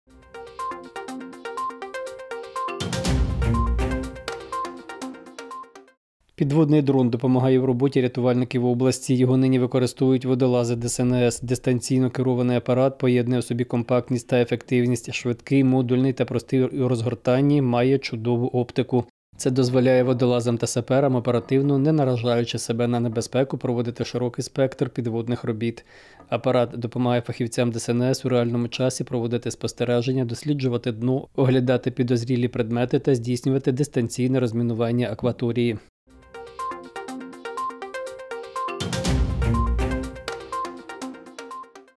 Черкаські рятувальники почали використовувати підводний дрон
Підводний дрон допомагає у роботі рятувальників області. Його нині використовують  водолази ДСНС. Дистанційно керований апарат поєднує в собі компактність та ефективність. Швидкий, модульний та простий у розгортанні, має відмінну оптику.
Це дозволяє водолазам та саперам оперативно, не наражаючи себе на небезпеку, проводити широкий спектр підводних робіт.
Апарат допомагає фахівцям ДСНС в реальному часі проводити спостереження, досліджувати дно, оглядати підозрілі предмети та здійснювати дистанційне розмінування акваторії.